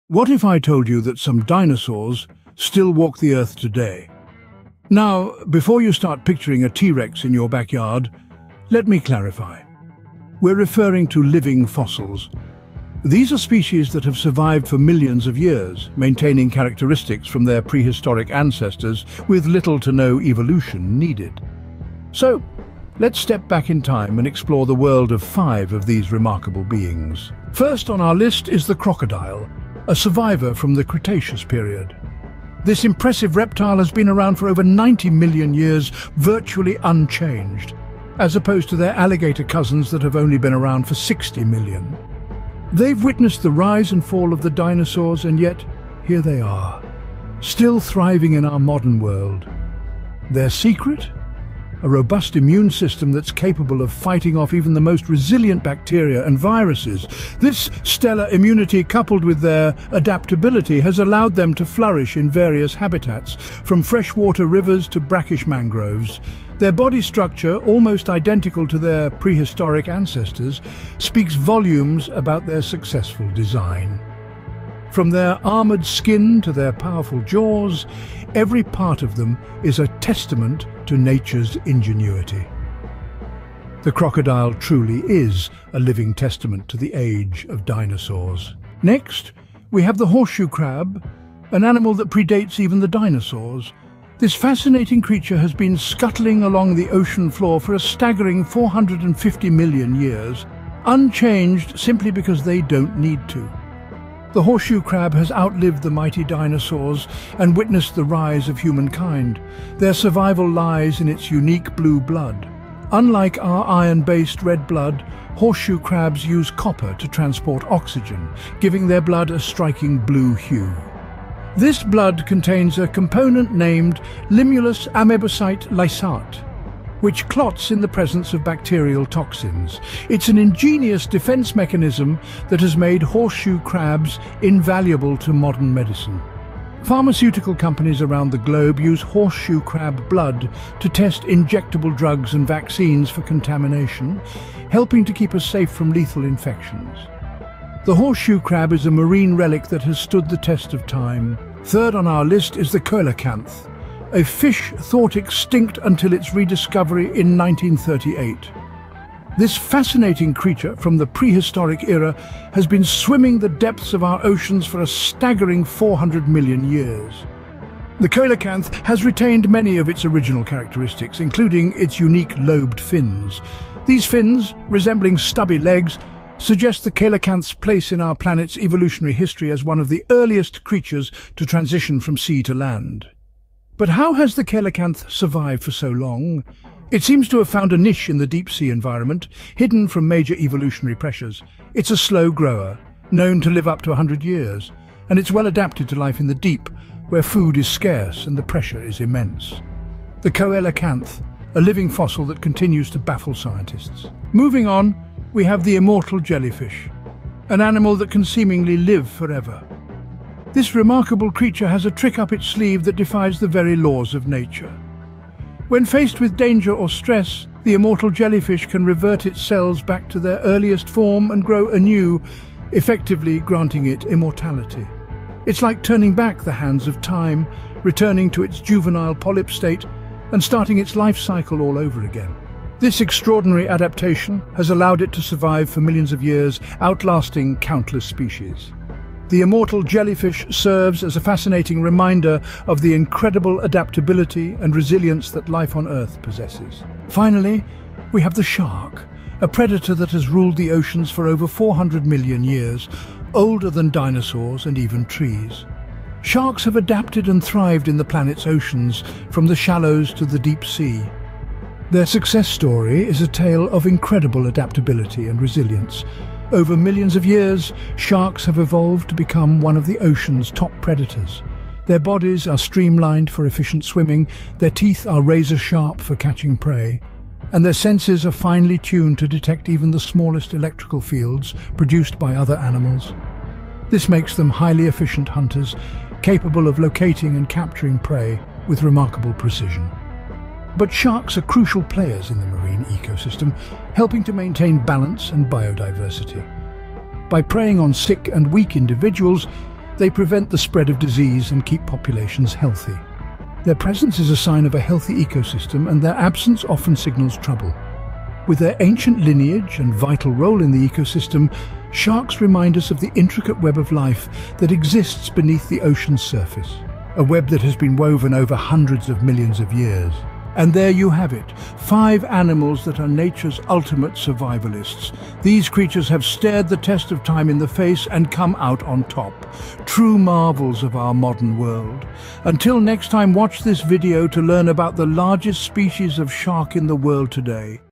5 Living Dinosaurs That Still Roam Earth Today! 🦖
5 Living Dinosaurs That Still Roam Earth Today! 🦖
In this captivating video, we embark on an extraordinary journey to explore the amazing world of living dinosaurs that still exist in our modern era. Join us as we uncover the hidden secrets and incredible facts about these prehistoric creatures that have managed to survive against all odds. From colossal sea monsters to majestic flying reptiles, get ready to witness the awe-inspiring diversity and adaptability of these living fossils. Our team of experts will share the latest scientific discoveries and theories, bringing you up-close encounters with these magnificent creatures. So buckle up and prepare to be amazed as we delve into the fascinating world of living dinosaurs. Let's embark on this adventure together!

OUTLINE:

00:00:00 Living Dinosaurs Among Us
00:00:27 The Mighty Crocodile
00:01:28 The Ancient Horseshoe Crab
00:02:31 The Ageless Coelacanth
00:03:24 The Immortal Jellyfish
00:04:06 The Timeless Shark
00:05:20 Conclusion